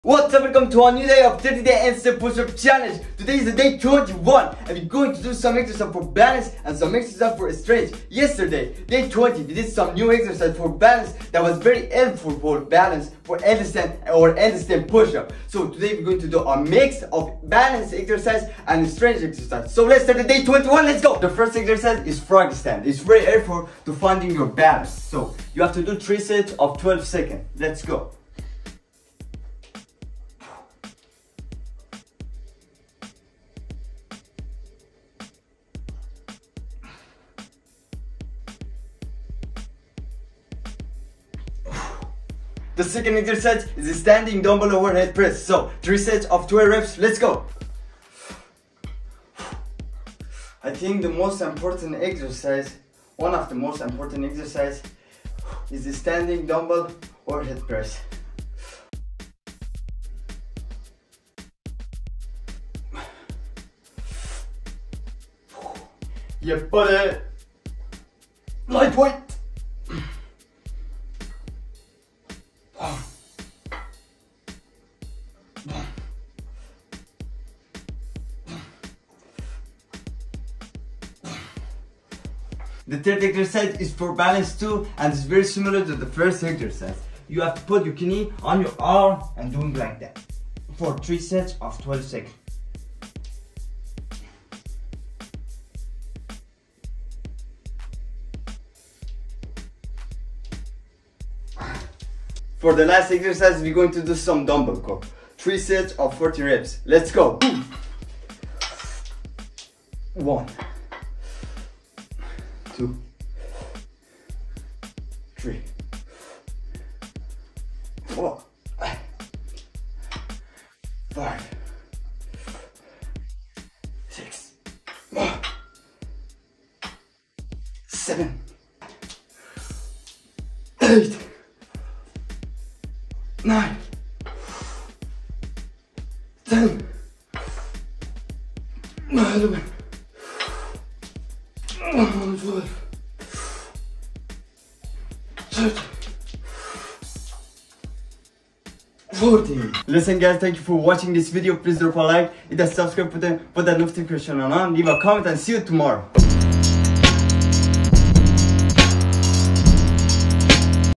HANDSTANDS Push Ups Challenge A game-changer For Your Life (21/30 DAY)
through the 30-Day Handstand Push-Up Challenge and guess what? Day 21 is here, and I'm rocking it! 🎉💪 I DID my first HANDSTAND push up! 🔥🙌